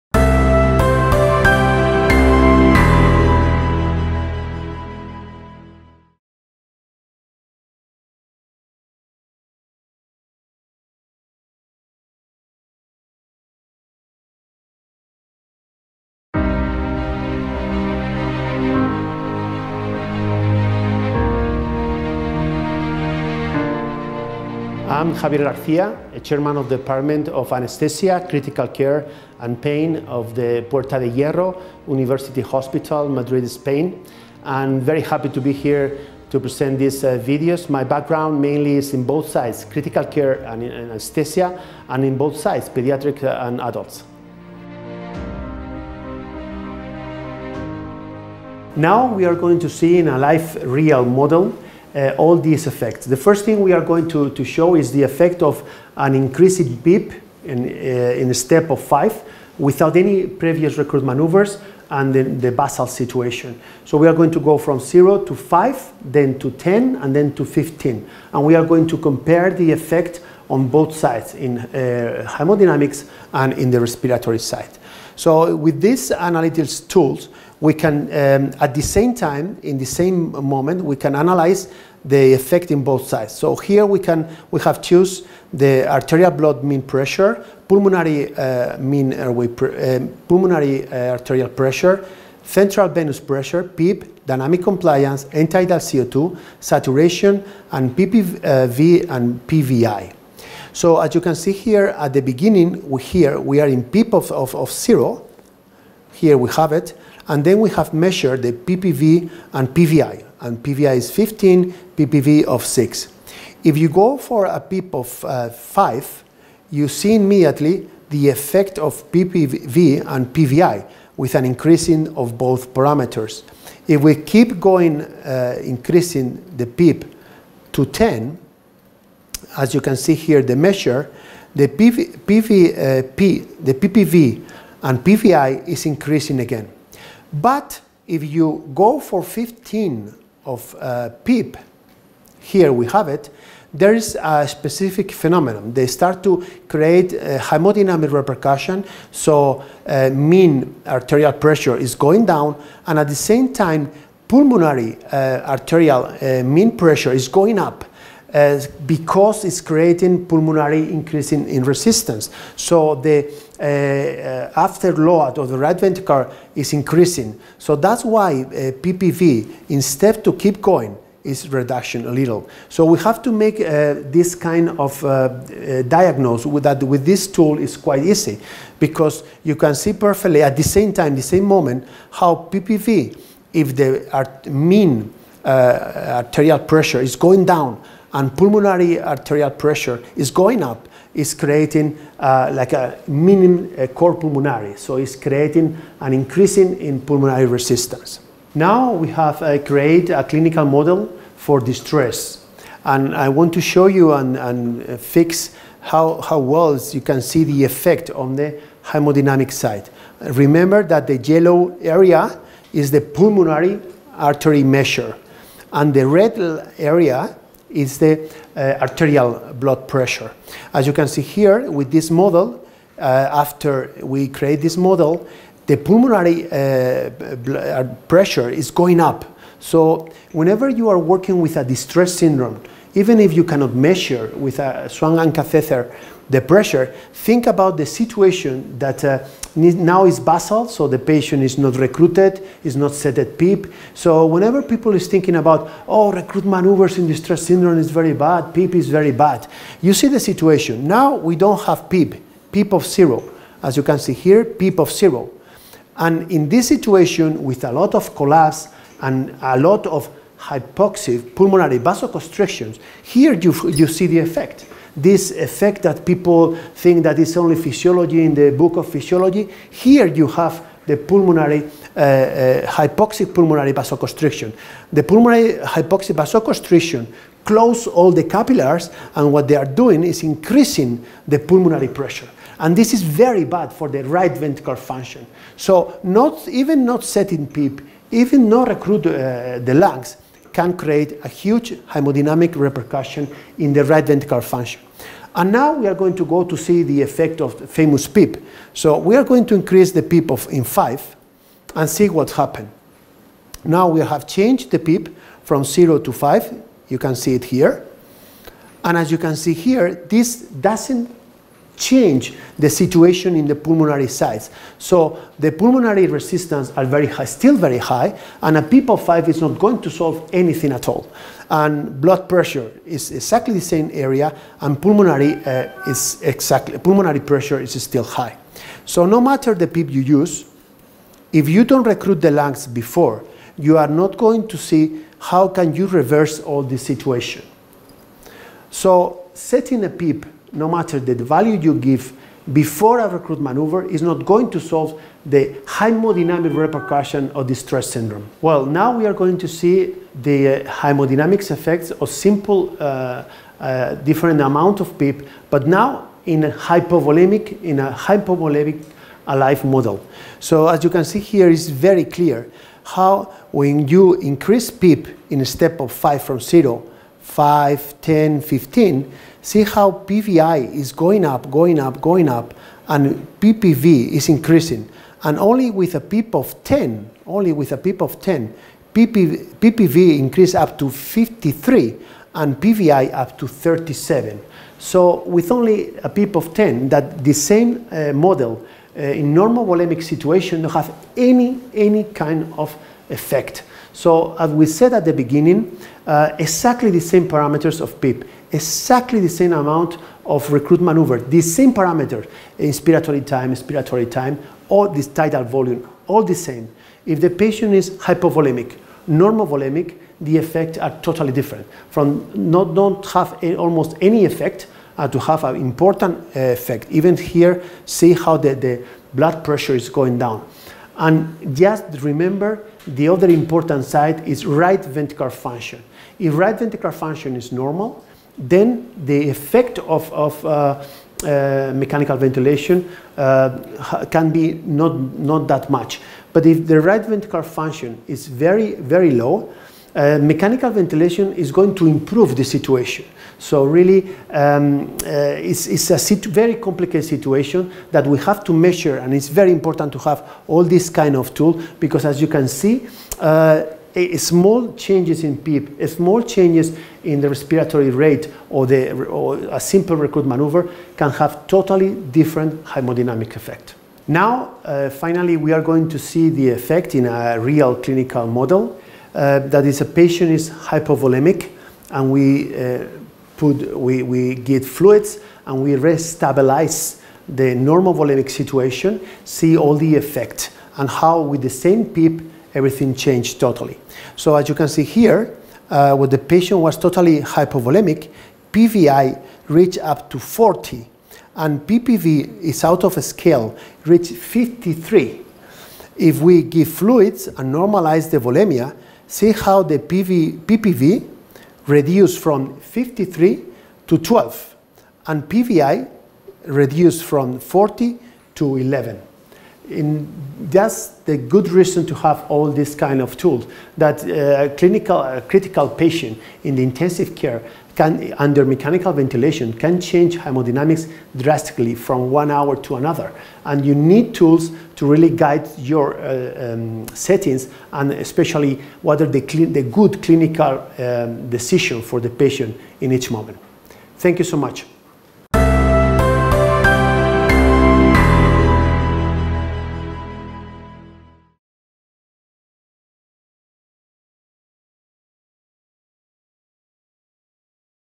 What are the main effects the mechanical ventilation is generating in hemodynamic
This video present in a clinical model the main effects in the hemodynamic of the mechanical ventilation in a acute patient under mechanical ventilation. In the ICU environment as well as in the OR. Dr. Javier García, head of Anaesthesia department in University hospital explains how the correlation between these correlation, and how the caregiver can improve the patient therapy based in this correlation.